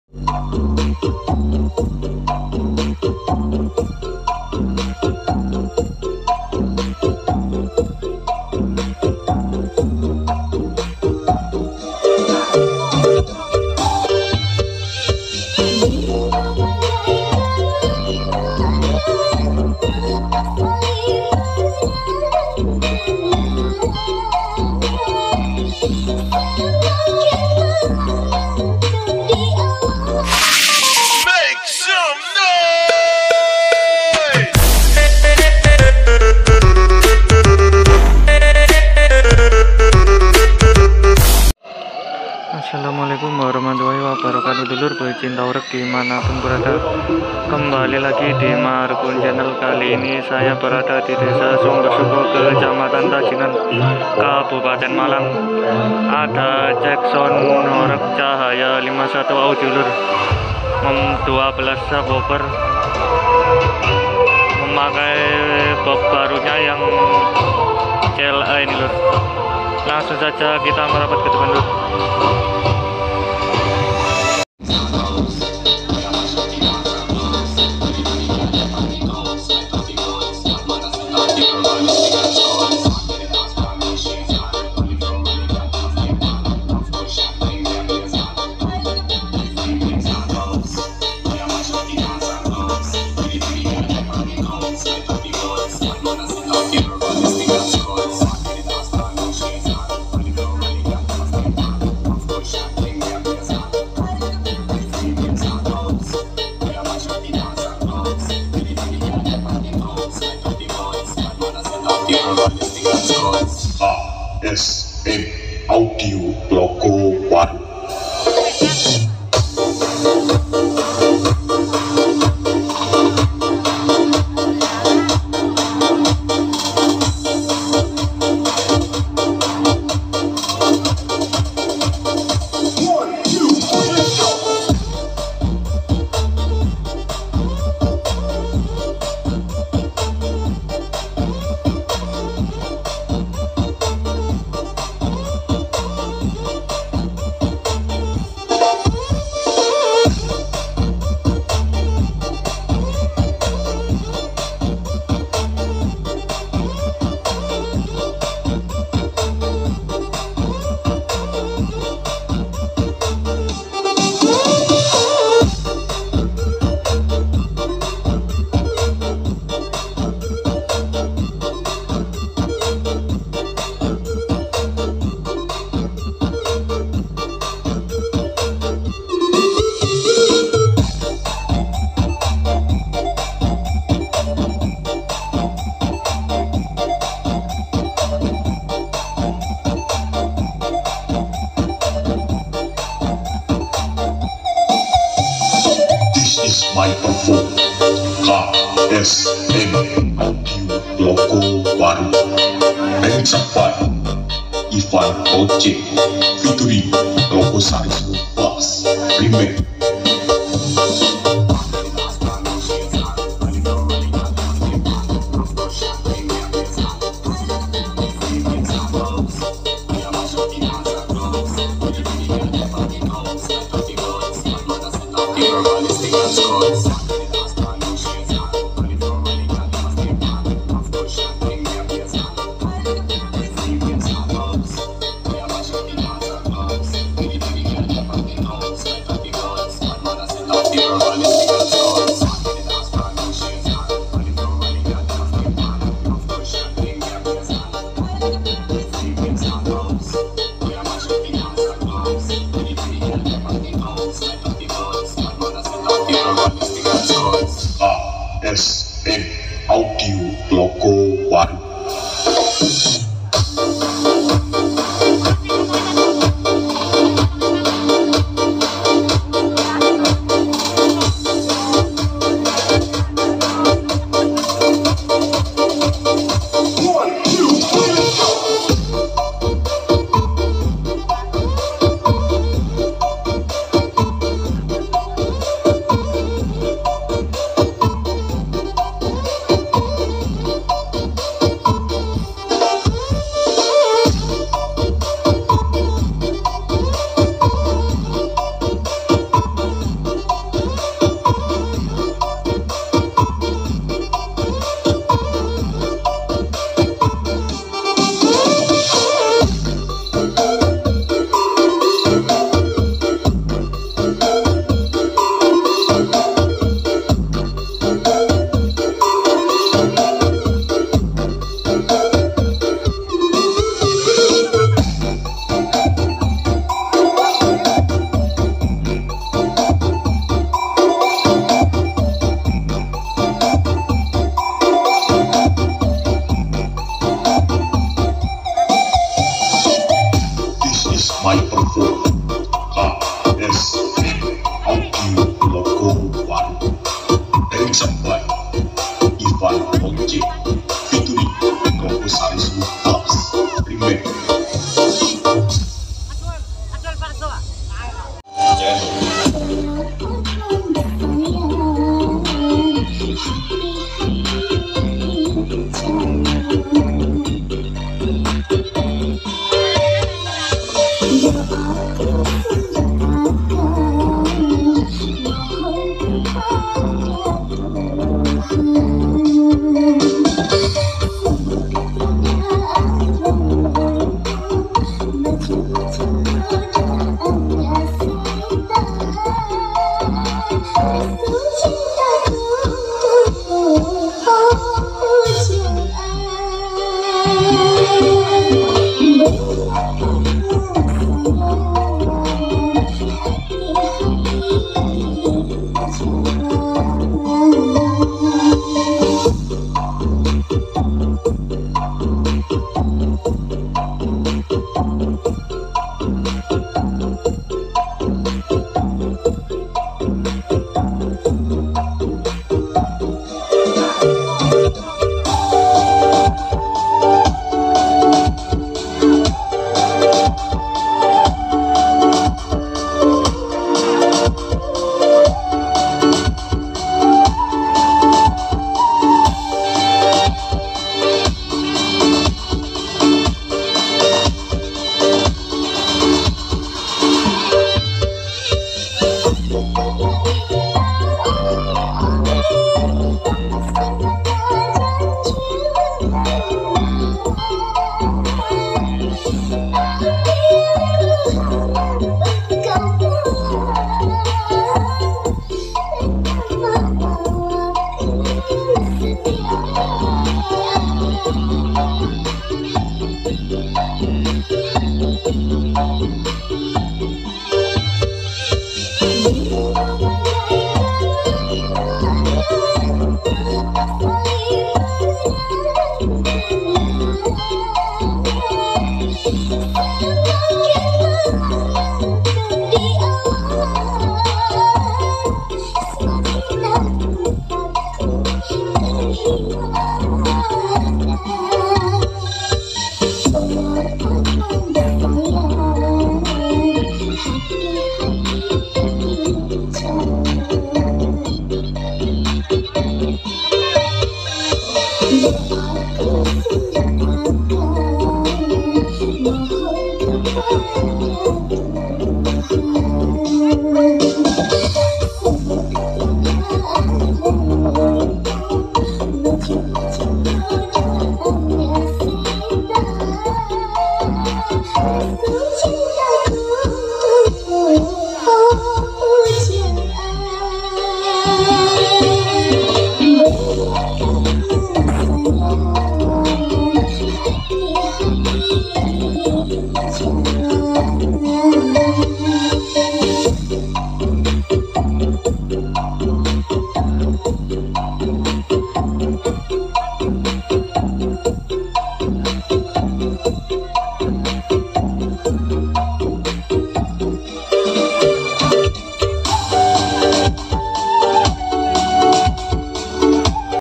Cek Sound CAHAYA 51 AUDIO di Desa Sumbersuko Tajinan Malang
assalamualaikum wr.wb
kembali lagi di Markon Channel kali ini ada Cek sound cahaya51 audio lor di desa Sumbersuko Tajinan Malang . membawa 12 subwofer ada box cla barunya 4 box

selamat menyaksikan semoga terhibur dan trimakasih atas supportnya 🙏